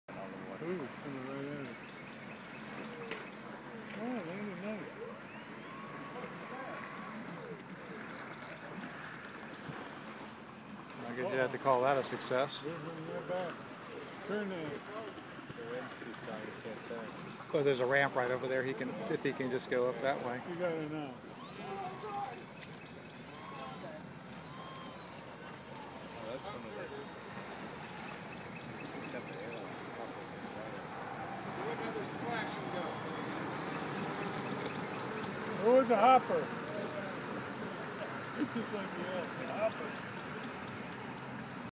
Slowboat Video 2 by Butch Tozier
Larry Fortna maidens new wing tip floats on his SlowBoat RC plane